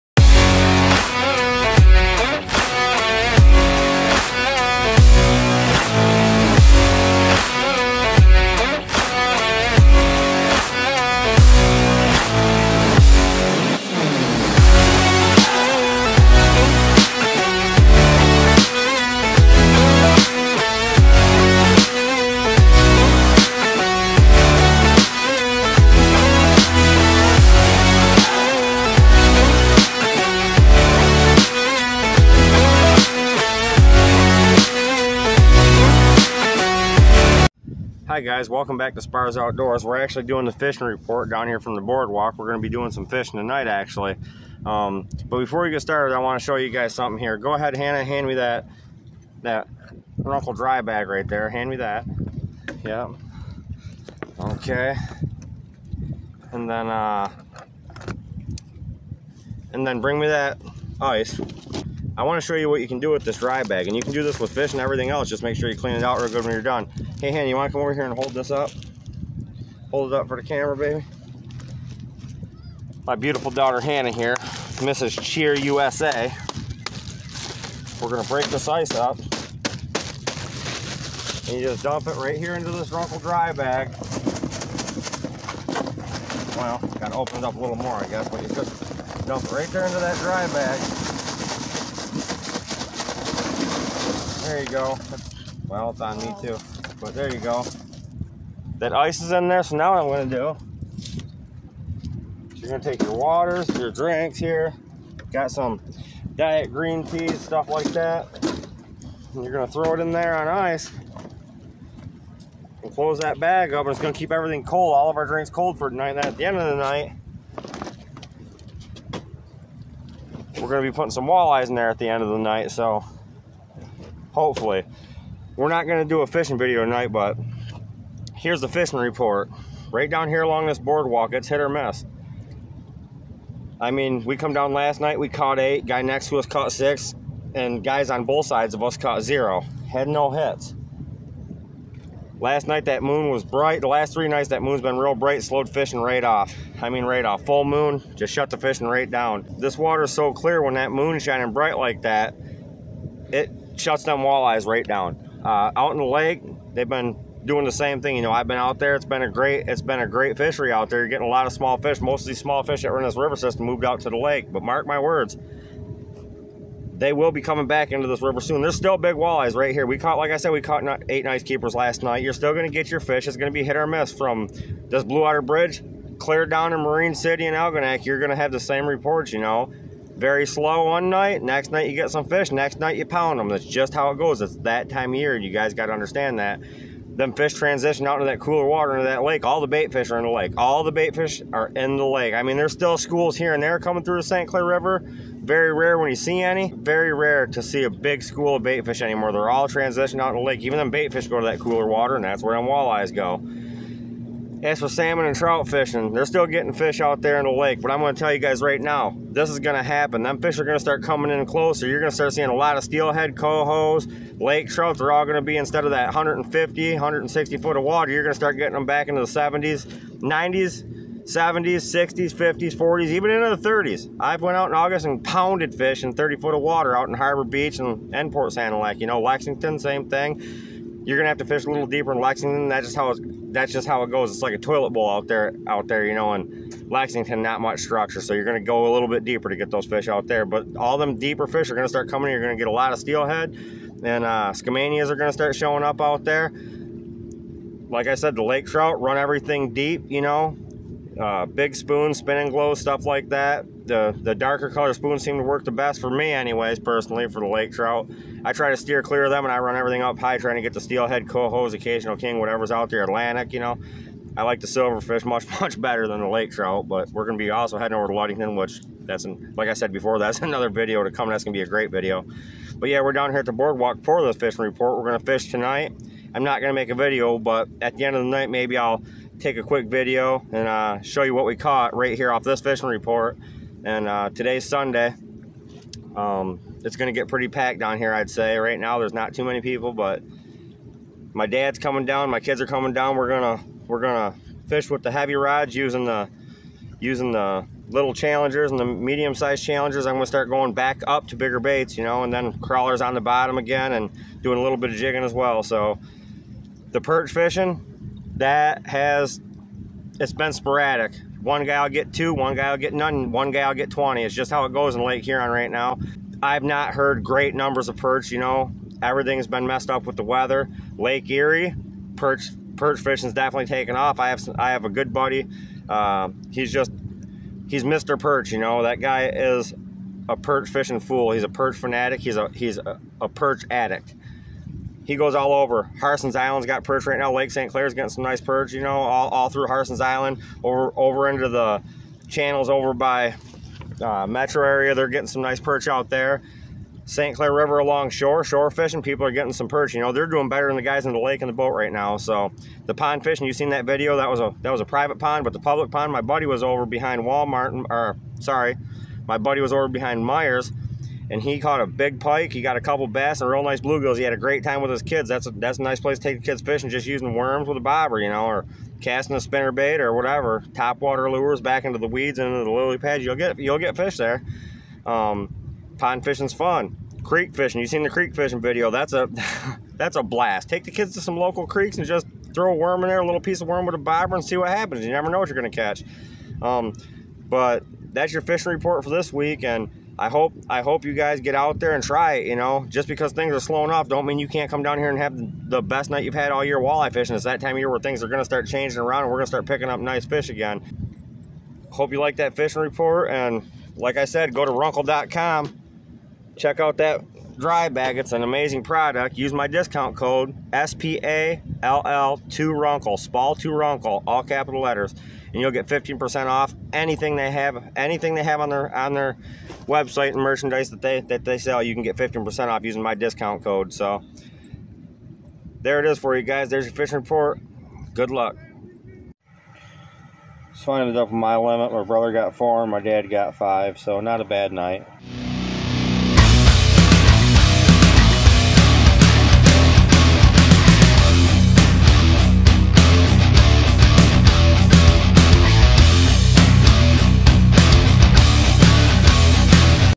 ST CLAIR RIVER/ LAKE HURON FISHING REPORT SUNDAY JULY 17 - 2022 WALLEYE, SALMON, PERCH ETC .
hope you enjoy the fishing report..